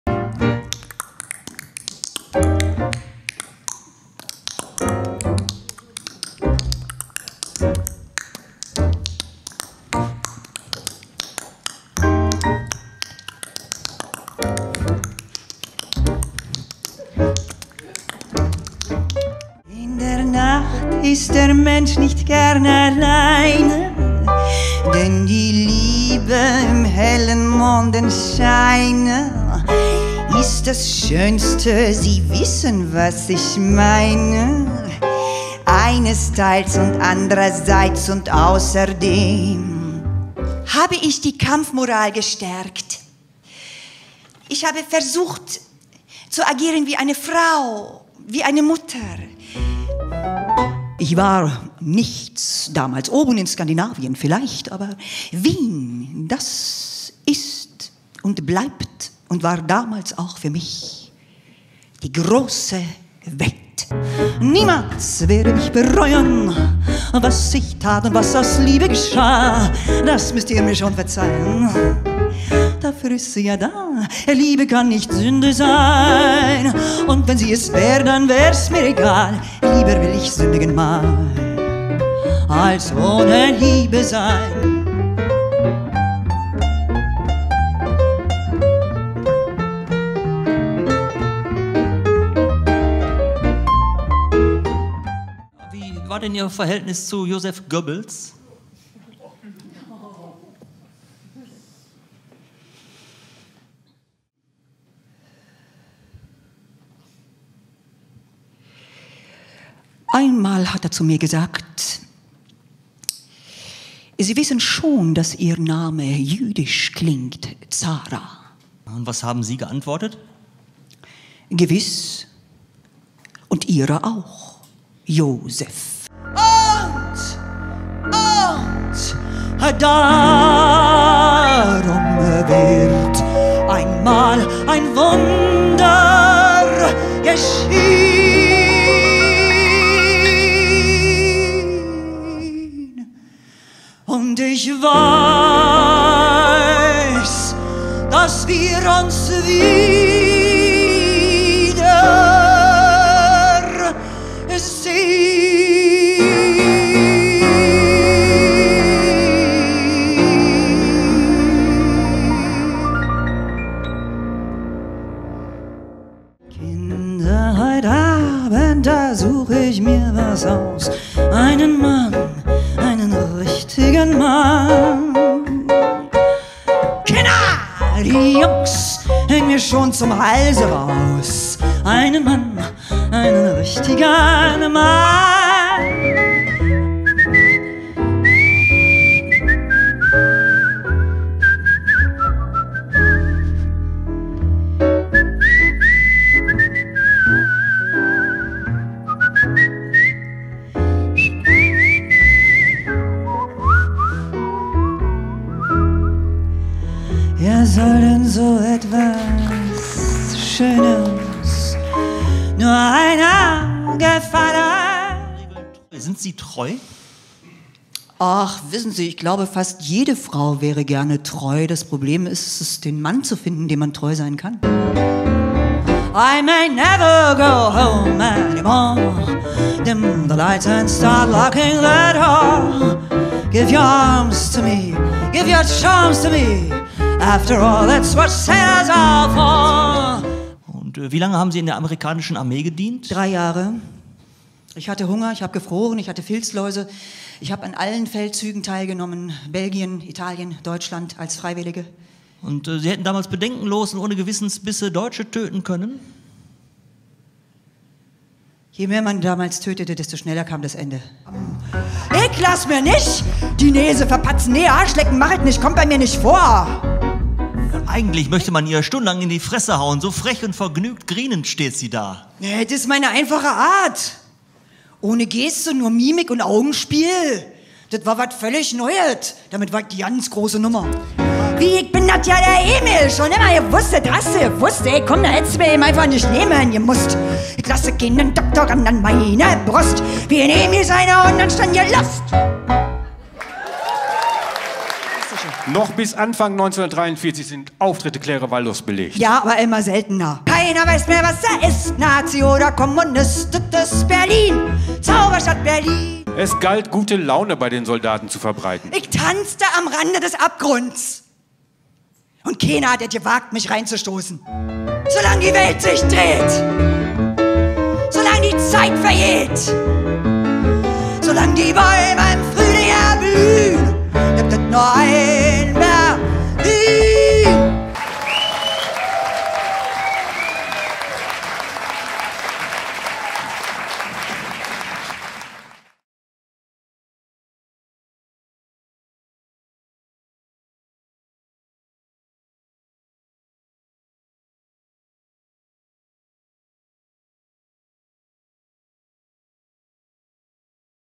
CONJAK "Diva. Macht. Mut." Trailer live
CONJAK
"Diva. Macht. Mut."
Marlene Dietrich, Zarah Leander, Marika Rökk und Claire Waldoff zwischen Widerstand und Opportunismus

In wie weit waren deutsche Diven in den 30er und 40er Jahren Meinungsmacherinnen? Warum ließen sich die Einen von den Machthabern instrumentalisieren und die Anderen nicht?
Ist es wirklich nur eine Frage des Mutes oder des Charakters? Wie viel Verdrängung ist nötig, um unter einem Unrechtsregime weiter arbeiten zu können? Und beeinflusst der Grad der moralischen Integrität den Wert des künstlerischen Schaffens?  Diesen Fragen spüren Christiane Hagedorn (Gesang), Martin Scholz (Piano) und Dieter Kuhlmann (Bass) ohne moralischen Zeigefinger nach und präsentieren einen musikalisch-komödiantischen Abend über vier außergewöhnliche Frauen, die zwischen Pandemie, Wirtschaftskrise und Weltkrieg zu Ikonen für Millionen wurden und im Sturm der Geschichte vor allem eines sein wollten: Künstlerinnen.  Christiane Hagedorn singt nicht nur ihre Lieder, mit wandelbarer, facettenreicher Stimme und feinem musikalischem Gespür, sondern lässt jede der Diven selbst zu Wort kommen, skizziert sie auf subtile Weise in kleinen Szenen- basierend auf Originalzitaten aus Interviews und Memoiren- und hat dabei mit Martin Scholz und Dieter Kuhlmann nicht nur zwei virtuose Musiker, sondern auch fabelhafte Spielpartner an ihrer Seite.